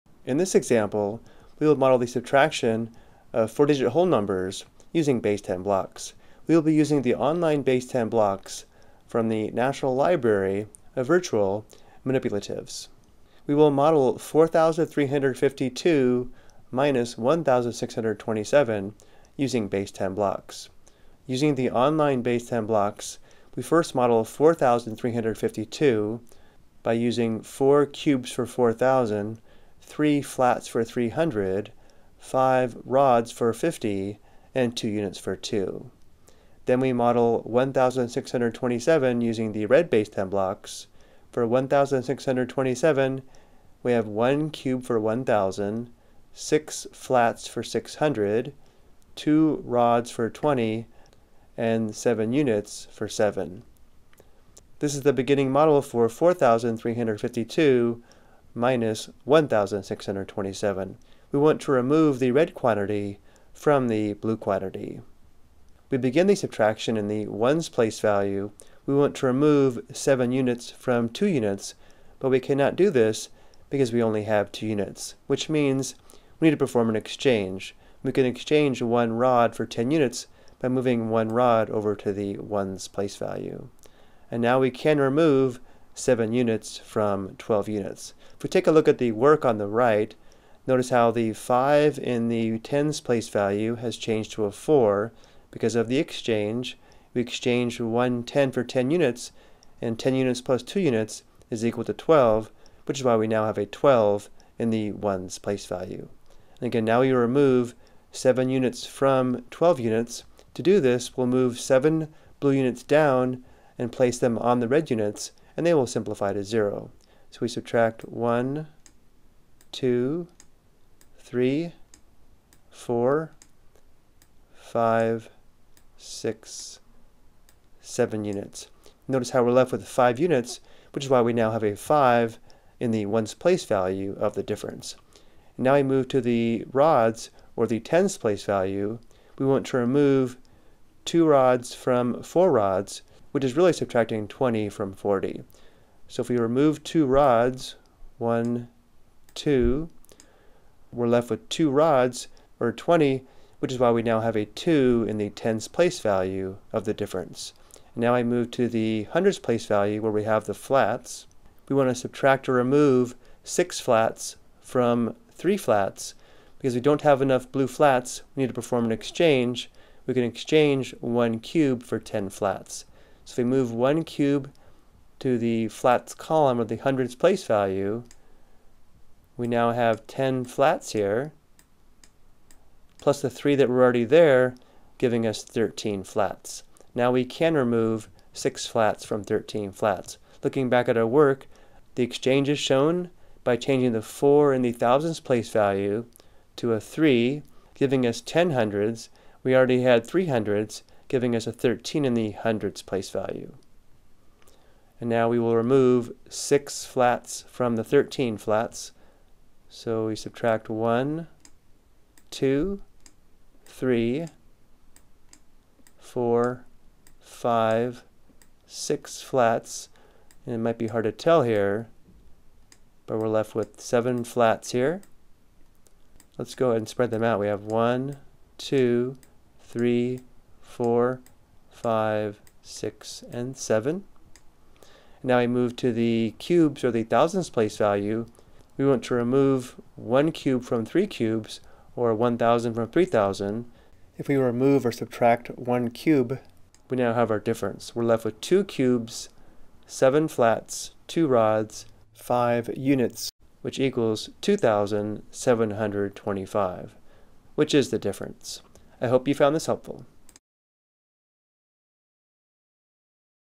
Subtract 4 Digit Whole Numbers Using Base 10 Blocks
The video explains how to model 4-digit  whole number subtraction using base 10 blocks.